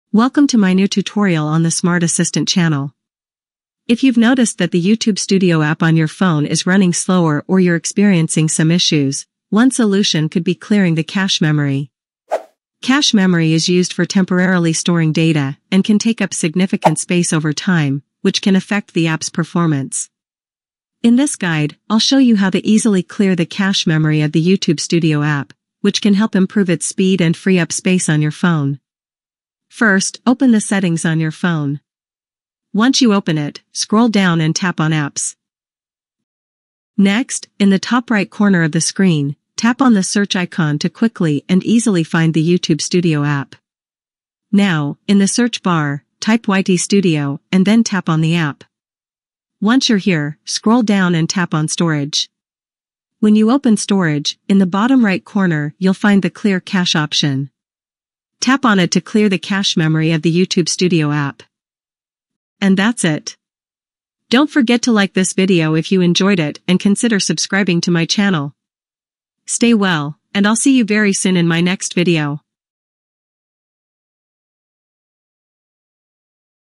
How to Clear the Cache Memory of the YouTube Studio App on a Samsung Galaxy Phone
If you've noticed that the YouTube Studio app on your phone is running slower or you're experiencing some issues, one solution could be clearing the cache memory. Cache memory is used for temporarily storing data and can take up significant space over time, which can affect the app's performance. In this guide, I'll show you how to easily clear the cache memory of the YouTube Studio app, which can help improve its speed and free up space on your phone.